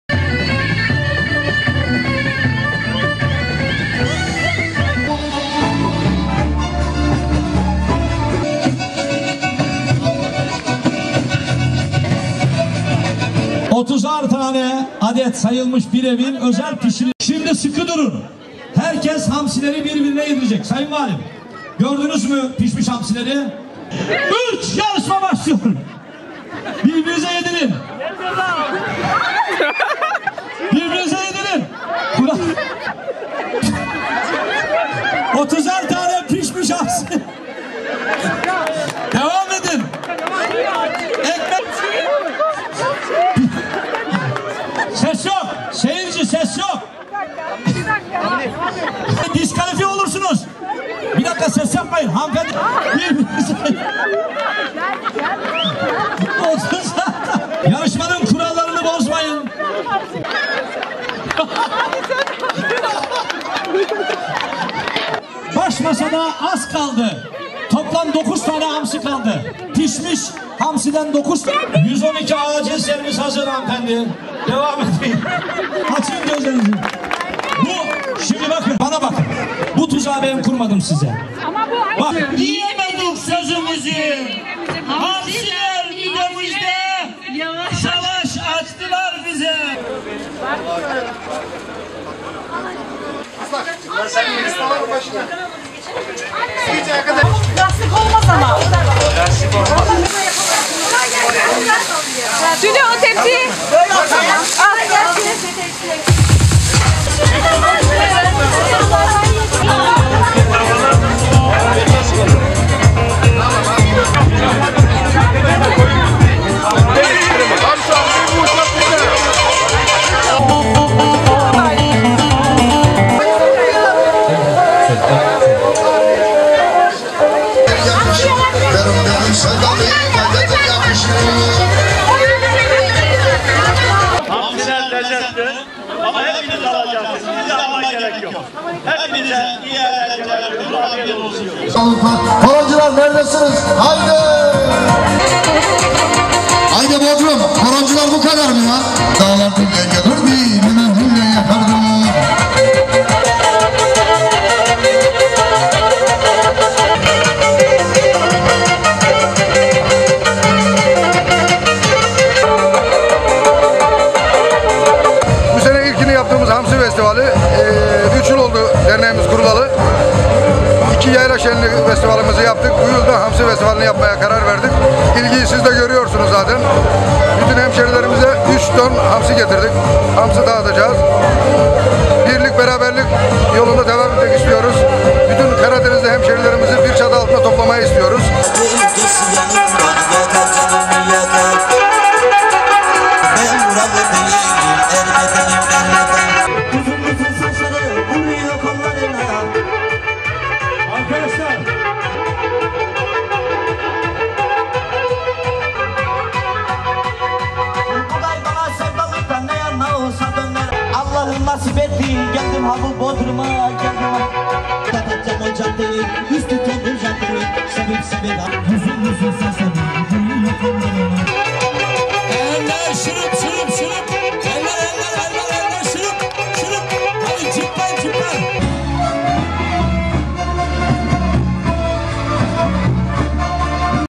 Anadolu Ajansı - Gözleri bağlı çiğ hamsileri yediler
Muğla'nın Bodrum ilçesinde bu yıl ilki düzenlenen festivalde pişirilen 3 ton hamsi dağıtıldı, çiğ hamsi yeme yarışması düzenlendi